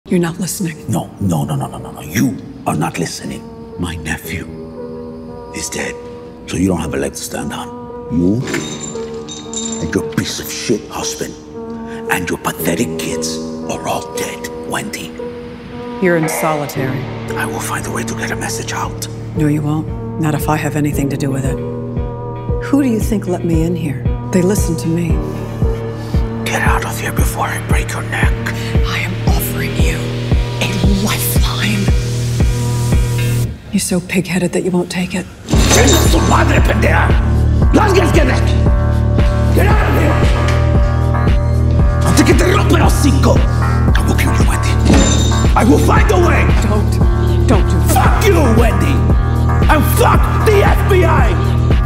Navarro Screams At Wendy🥶 //
In this video, Wendy visits Navarro in prison, and they are talking about Javi. Navarro then tells Wendy that she doesn't have any leverage, but Wendy says something that causes Navarro to scream.